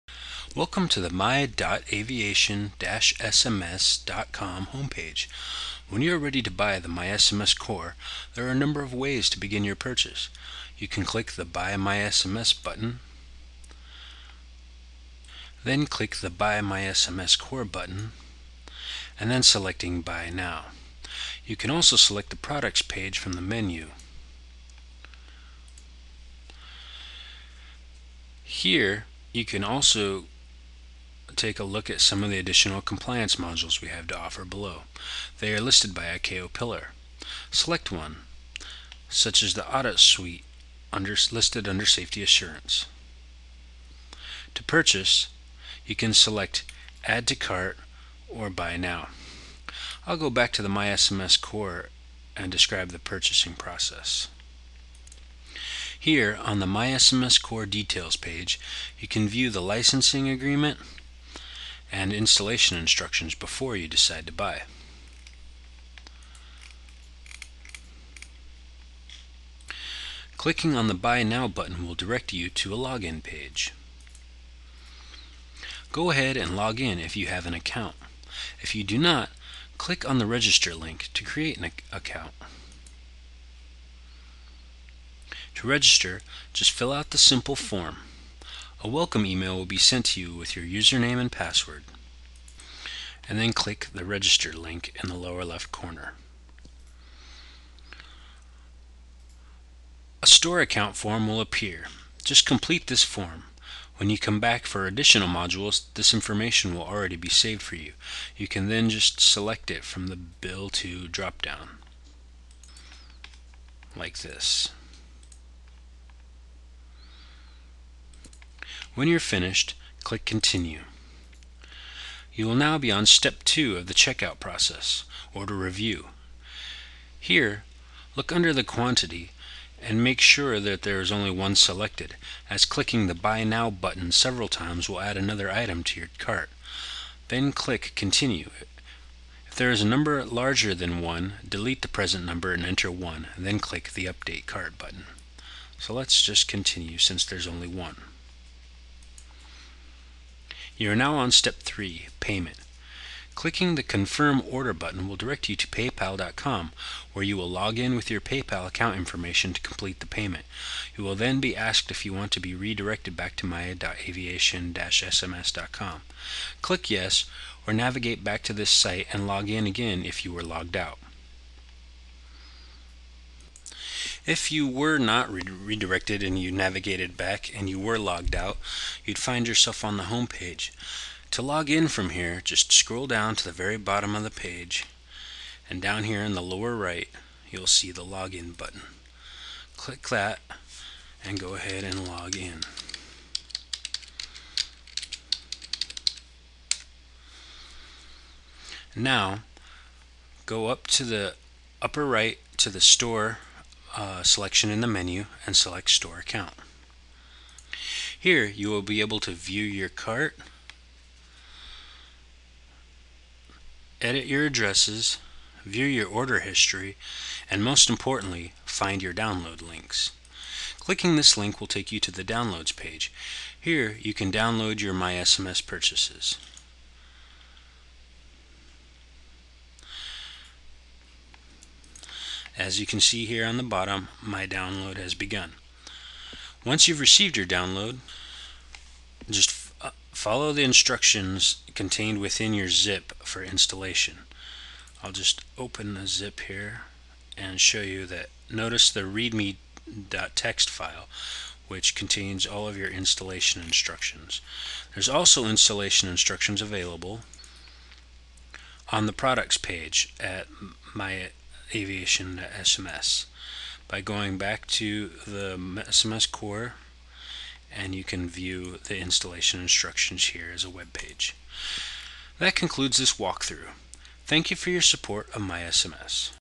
mySMS - How to buy & build your own SMS system for aviation industry using mySMS modules Video
Based on your needs and the maturity level of your SMS program, you can build your own system using mySMS modules that have been developed specifically for aviation industry.

Your System, Your Data Security, Customized Your Way !

mySMS focuses on the four ICAO SMS pillars

1. Safety Policy
2. Risk Management
3. Safety Assurance
4. Safety Promotion

6 REASONS TO CHOOSE mySMS

1. Security - Your sensitive data remains in-house
2. Anywhere, Anytime (+Anyone) - Next generation WWW Application
3. Low Cost - No recurring support fees
4. Worldwide Support - World class 24/7/365 LIVE support
5. Facilitates ICAO Compliance - Based on ICAO SMS four pillars
6. Excellent Features - 50+ modules built on popular open source framework